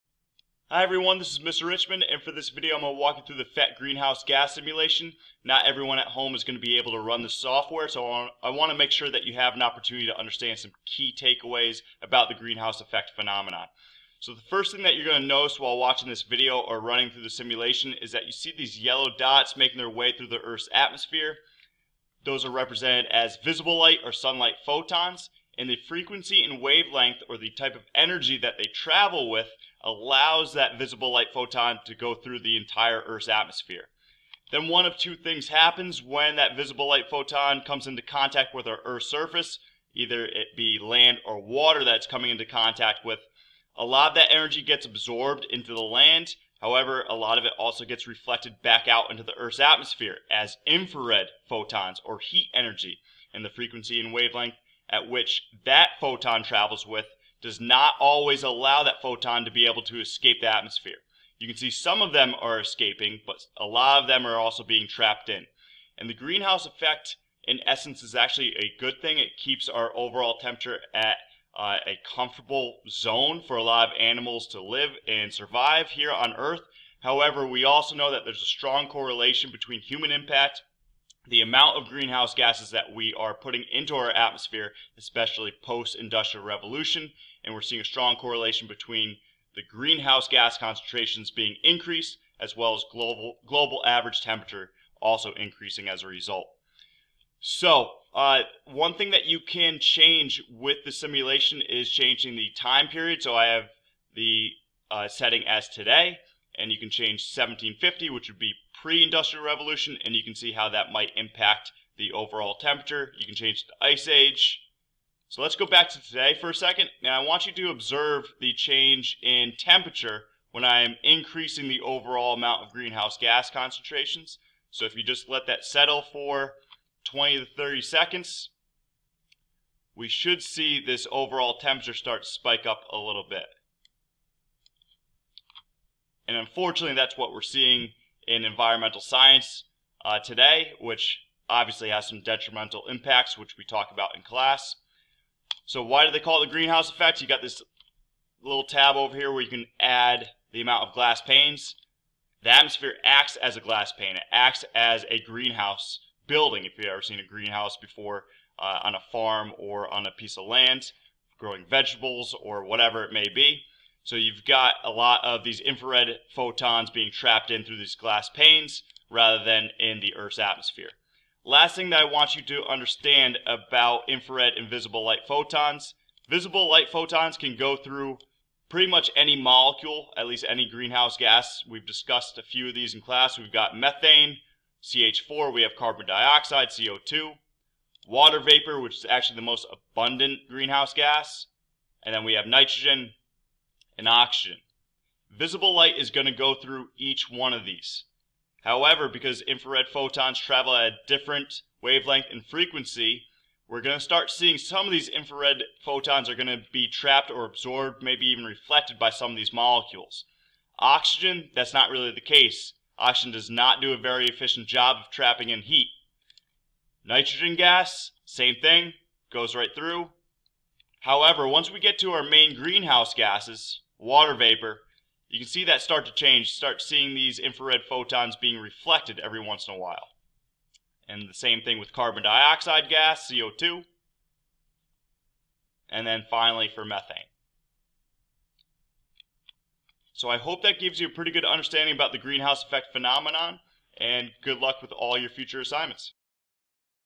Greenhouse Effect Explained Using Phet
Brief description of the Greenhouse effect for student use.